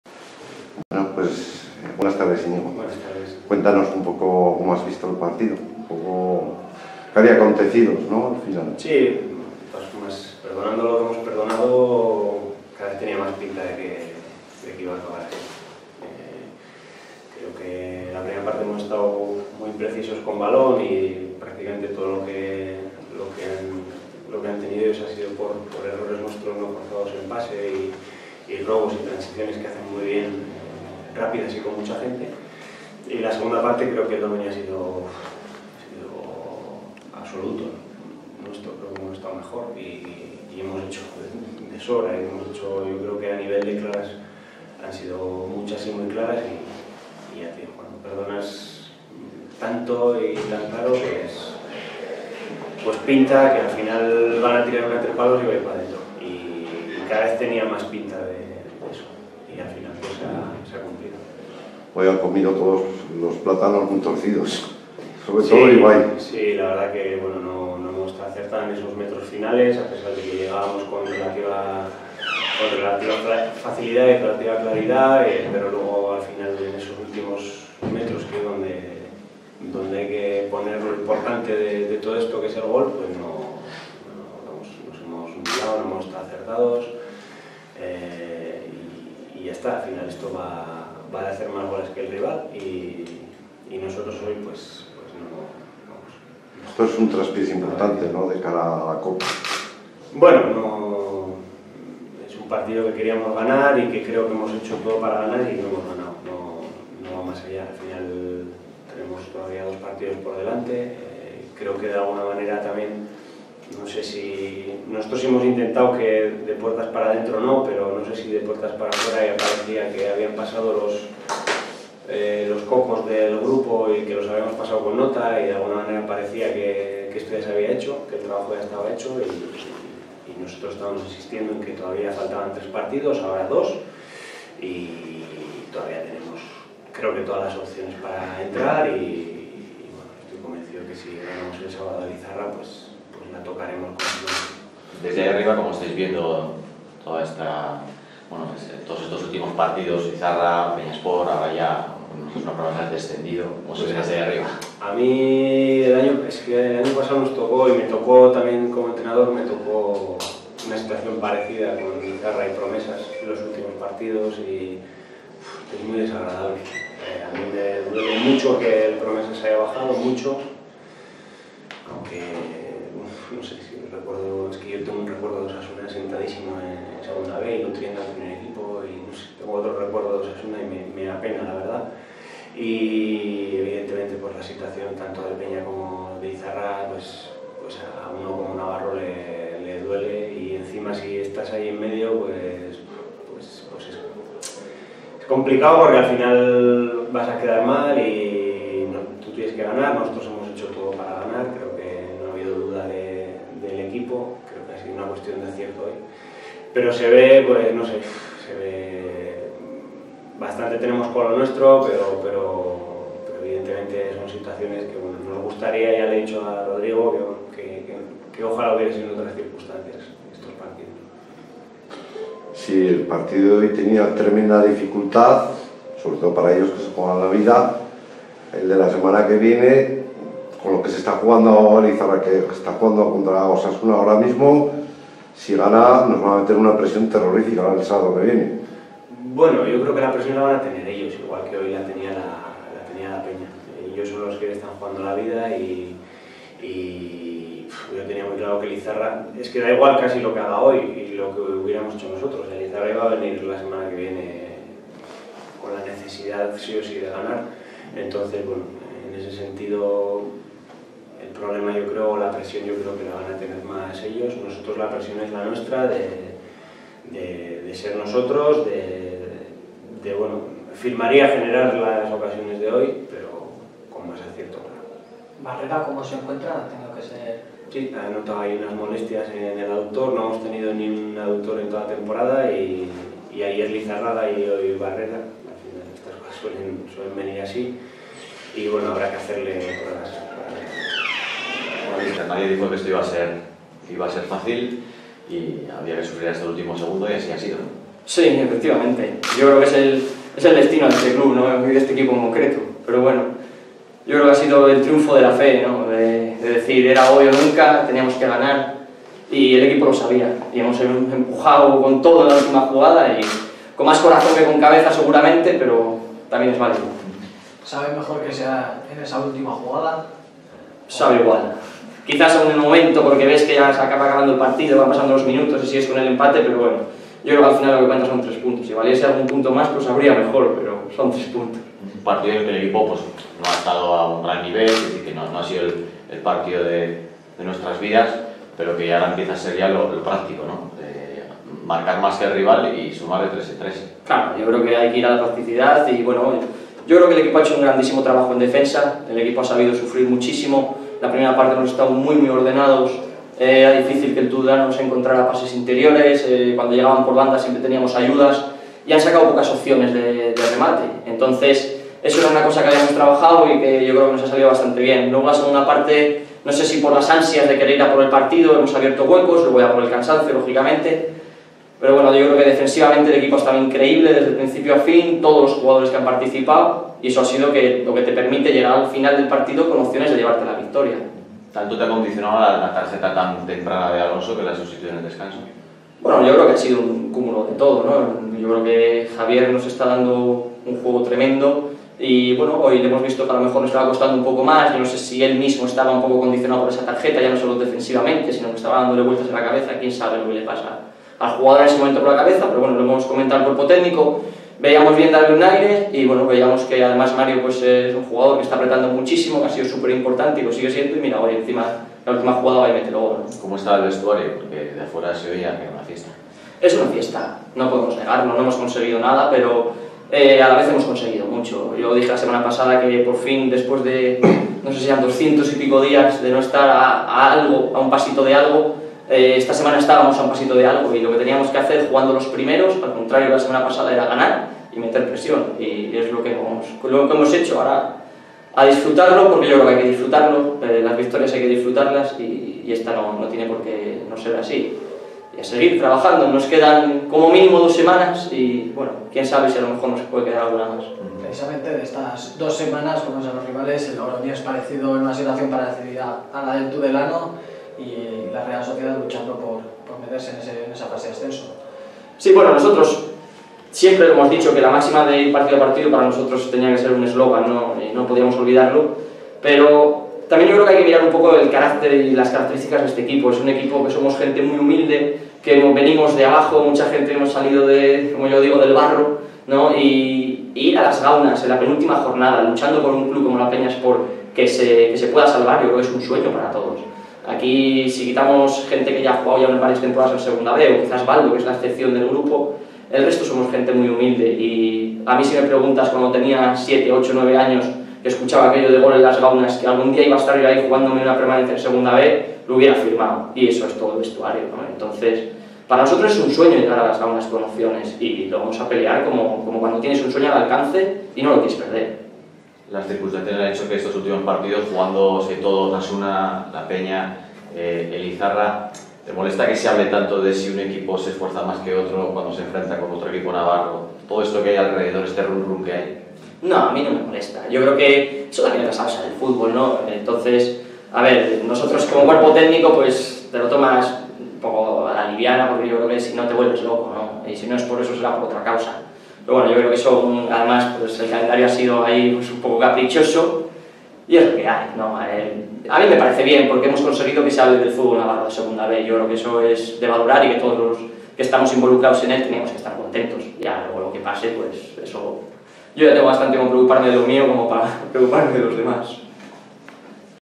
Peña Sport 1-0 Tudelano. Declaraciones de Valencia y Hernando
Declaraciones de Íñigo Valencia, entrenador del CD Tudelano, y de Rodrigo Hernando, entrenador de la Peña Sport FC, tras el Peña Sport 1-0 Tudelano, correspondiente a la jornada 36 del Grupo II de Segunda División B, de la temporada 2017/2018, disputado el 28 de abril de 2018.